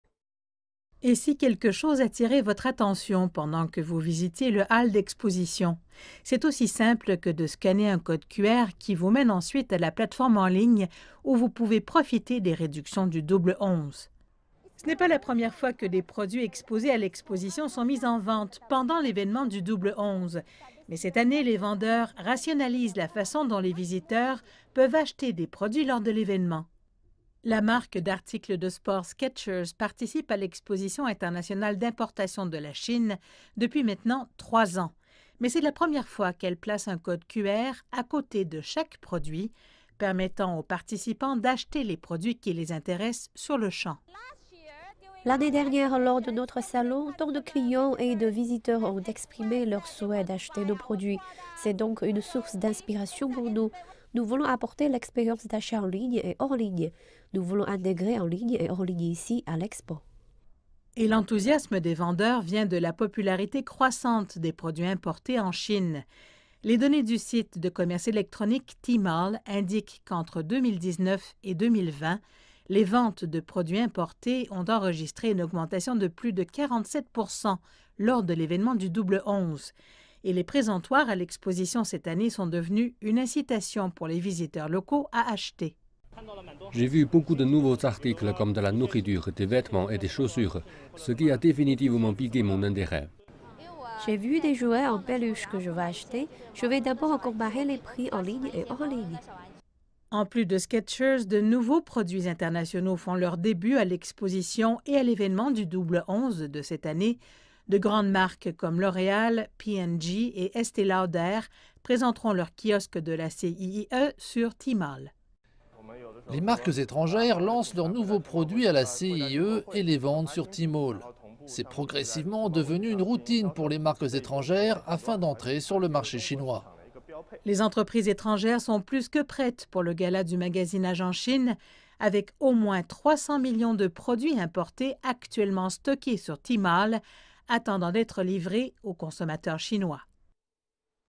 CIIE : les exposants prêts pour le "Double 11"
Double 11, le plus grand événement commercial au monde débutera après le dernier jour de l'Exposition internationale d'importation de la Chine. Et cette année, les exposants cherchent à rendre l'expérience d'achat encore plus pratique en ligne et hors ligne.